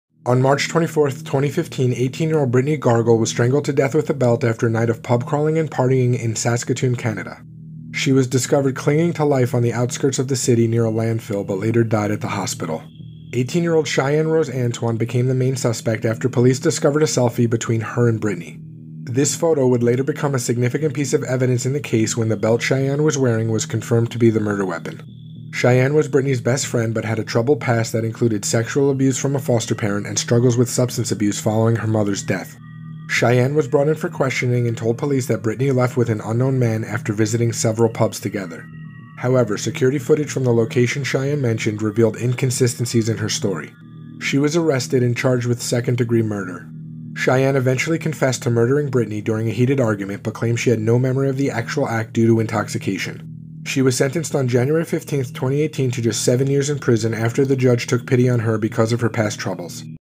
MURDERED: Brittney Gargol
In the bustling streets of Saskatoon, Canada, a friendship between Brittney Gargol and Cheyenne Rose Antoine flourished, despite the shadows cast by Cheyenne’s troubled past. This past was marked by a string of personal struggles, including the trauma of foster care abuse and the heartache following her mother's death. These hardships fueled Cheyenne's descent into substance abuse, a path that would eventually intertwine with a fateful night of pub crawling and partying.

On March 24, 2015, the city's vibrant nightlife was a backdrop for the duo's escapade, an evening that began with laughter and camaraderie but ended in an unimaginable tragedy. As the night wore on, the youthful energy that fueled their adventure turned dark. In the early hours, amidst the Canadian winter, Brittney was found clinging to life on the outskirts of Saskatoon, near a landfill. Despite the efforts of the first responders, she succumbed to her injuries in the hospital, leaving behind a trail of questions and a community in shock.

The investigation into Brittney's death unfolded like a complex puzzle. The police, armed with their knowledge of criminal psychology and their experience in handling cases of violent crime, sifted through the evidence. Central to their inquiry was a selfie, a digital footprint of the evening's events, capturing Brittney and Cheyenne, with Cheyenne donning a belt that would later be identified as the murder weapon.

Initially, Cheyenne spun a tale of deception, pointing to an unknown man as the perpetrator. However, inconsistencies in her story, highlighted by security footage from the locations she mentioned, steered the suspicion towards her. The investigation's momentum picked up, leading to Cheyenne's arrest and charge of second-degree murder.

As the case unraveled in the judicial system, it painted a picture of a friendship turned betrayal. Cheyenne's confession to murdering Brittney during a heated argument came as a shock. Her claim of having no memory of the act due to intoxication added another layer of complexity to the already intricate case.

In the courtroom, the narrative of Cheyenne's life - marked by youth and crime, personal struggle, and the quest for a semblance of normalcy despite her upbringing - evoked a sense of judicial leniency. The judge, taking into account her past troubles and the circumstances leading to the crime, sentenced her on January 15, 2018, to just seven years in prison. This sentencing sparked a controversy, leaving the community and observers divided.

The case of Brittney Gargol and Cheyenne Rose Antoine remains a poignant reminder of how quickly lives can be altered forever. It underscores the critical role of law enforcement and the legal system in dissecting the layers of a crime scene investigation. It speaks to the influence of personal struggles on one's actions and the unpredictable nature of human relationships. Above all, it's a tragic tale of a friendship that ended in a most unexpected and heartbreaking way, leaving a permanent imprint on the city of Saskatoon and beyond.